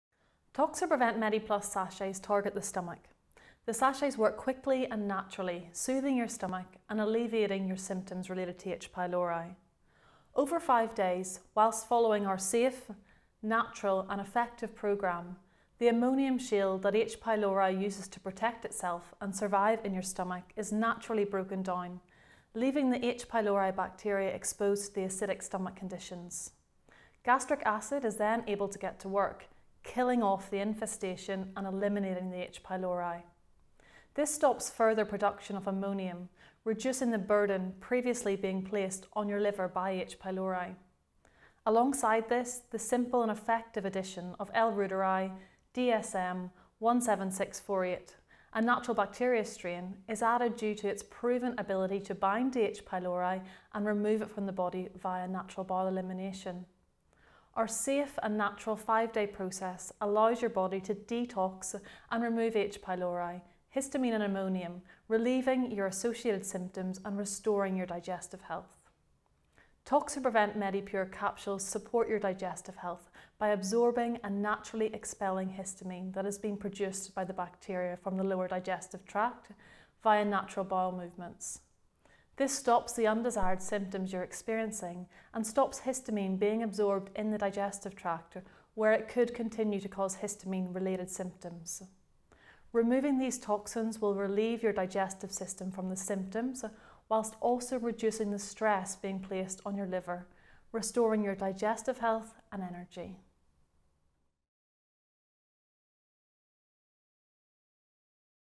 How Toxaprevent Medi Plus Sachets and L. Reuteri DSM 17648 capsules detoxes helicobacter pylori
The signs or symptoms that come with H. pylori infection can be severe, and you may feel run down, exhausted and the feeling of nausea can be overwhelming.

1)An ache or burning pain in your abdomen
2) Abdominal pain that's worse when your stomach is empty
3) Nausea
4) Loss of appetite
5) Feeling full quickly
6) Frequent burping
7) Bloating
8) Unintentional weight loss

Infection with H. pylori is common. About two-thirds of the world’s population has it in their bodies. H Pylori is a stomach bacteria that can enter your body and live in your digestive tract.

Toxaprevent Medi Plus Sachets target the stomach. The sachets work quickly and naturally, soothing your stomach and alleviating your symptoms related to H. Pylori.

Over 5 days whilst following our safe, natural and effective programme, the ammonium shield that H. Pylori uses to protect itself and survive in your stomach is naturally broken down, leaving the H.Pylori bacteria exposed to the acidic stomach conditions. Gastric acid is then able to get to work, killing off the infestation and eliminating the H. Pylori.

This stops further production of ammonium, reducing the burden previously being placed on your liver by H. Pylori.

Alongside this, the simple and effective addition of L Reuteri DSM17648, a natural bacteria strain, is added due to its proven ability to bind to H Pylori and remove it from the body via bowel elimination.

Our safe and natural 5 day process allows your body to detox and remove H Pylori, histamine and ammonium relieving your associated symptoms and restoring your digestive health.

Presented by April Galbraith, Naturopathic Nutritional therapist & Medical thermographer BA, DipCNM, mANP from the Nouveau Healthcare team. Medically checked by Gaya Neault BSc Chemistry with Medicinal Chemistry.

Scientific References:

Detection of Helicobacter pylori with stool antigen test in children with gastroesophageal reflux disease. The American Journal of Gastroenterology; New York Vol. 96, Iss. 6,  (Jun 2001). Demir, Hülya; Ercis, Serpil; Koçak, Nurten; Hasçelik, Gülsen; Ozen, Hasan; et al.

Synopsis of the Polyvalent Qualities of Zeolite-Clinoptilolite and the Proposed Uses Within
the Namibian Medical, Pharmaceutical, Industrial and Economical Sectors: A Proposal for an
Integrated Research. Barbara Kahler. Pg 54-57

( also applies to histamine intolerance, allergies & toxicity/heavy metals)

Antibacterial activity of Zn-loaded Cuban zeolite against Helicobacter pylori in comparison to its Na-loaded and unmodified counterparts. Guido Cerri, Mauro Farina, Antonio Brundu, Elisabetta Gavini,  Andrea Salis, Wilfried Dathe
Antibacterial activity of Na-clinoptilolite against Helicobacter pylori: in-vitro tests, synergistic effect with amoxicillin and stability of the antibiotic formulated with the zeolite. MauroFarina, AntonioBrundu, Maria Cristina Bonferoni, ClaudiaJuliano, GiovannaRassu, ElisabettaGavi, Guido Cerr.
Histamine-binding capacities of different natural zeolites: a comparative study. Thangaraj Selvam, Wilhelm Schwieger, Wilfried Dathe.

Critical Review on Zeolite Clinoptilolite Safety and Medical Applications in vivo. Sandra Kraljević Pavelić, Jasmina Simović Medica, Darko Gumbarević, Ana Filošević, Nataša Pržulj and Krešimir Pavelić.